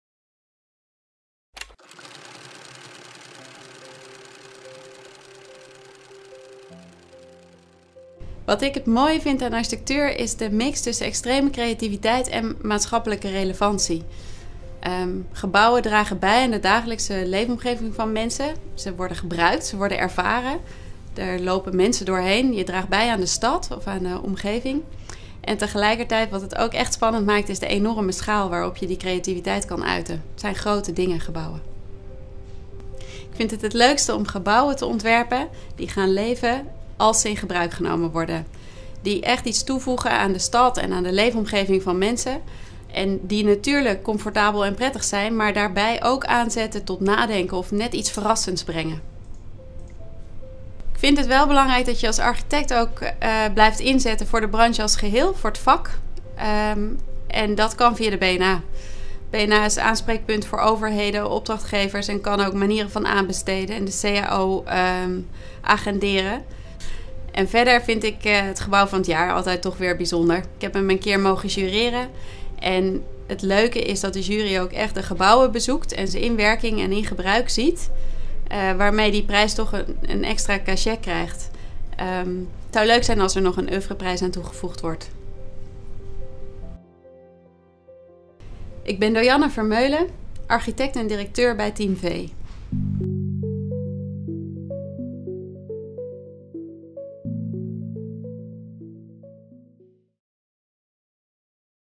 Het effect van Do Janne Vermeulen
In deze video vertelt Do Janne Vermeulen over haar effect als BNA Architect. Do Janne Vermeulen is architect-directeur en founding partner van Team V Architectuur Kijk voor meer informatie en alle ‘effect van een BNA Architect’ op